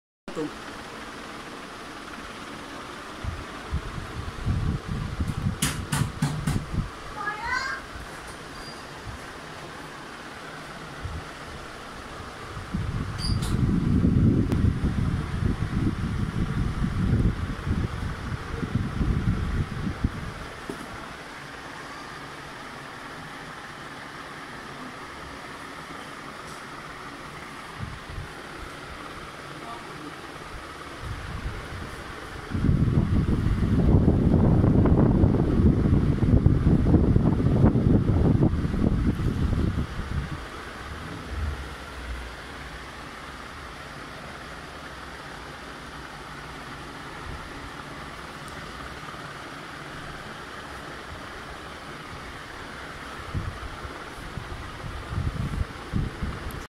Hijama Therapy for general refreshment and low back pain
One of the best regimen for many ailments, safe and effective Unani treatment